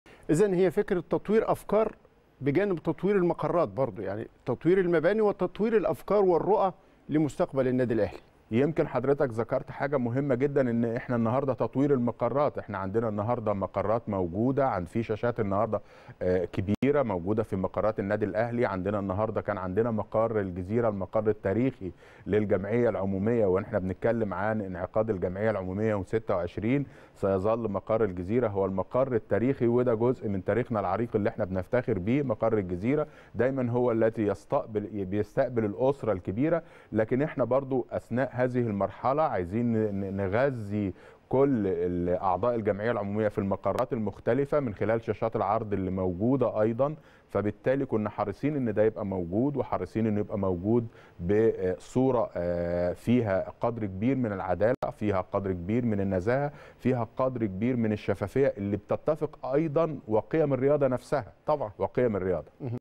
سعد شلبي: مقرات النادي الأهلي شهدت تطورا كبيرا
سعد شلبي: مقرات النادي الأهلي شهدت تطورا كبيرا | الأهلي ينتخب